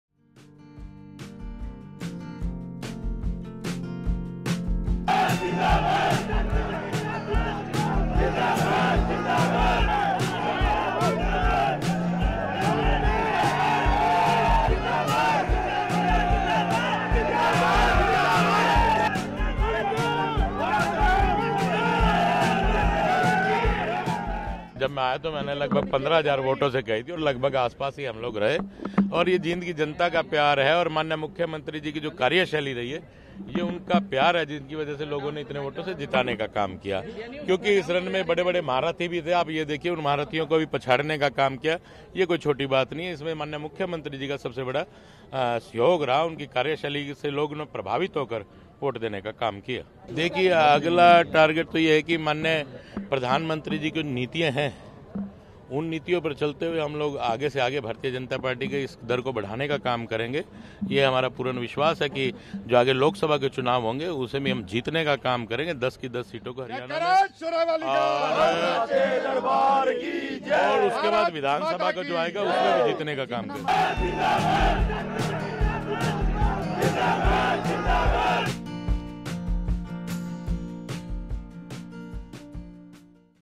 We will follow PM Modi's schemes and take them forward: Jind bypoll winner KL Middha - Haryana News
Jind (Haryana), Jan 31 (ANI): Bharatiya Janata Party's (BJP) candidate KL Middha won the Jind by elections with a comfortable margin today.